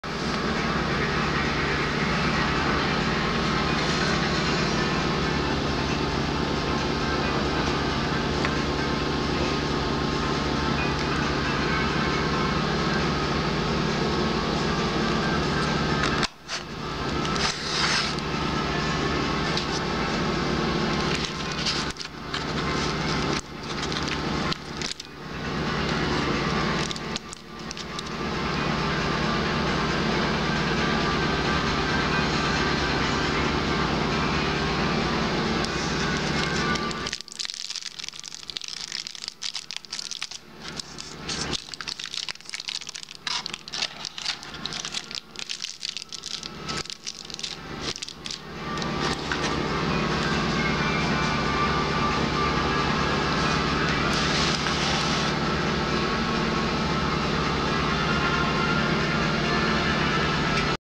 Латки для камеры RS36
Велосипедные латки в случае того,если вы пробили колесо на дороге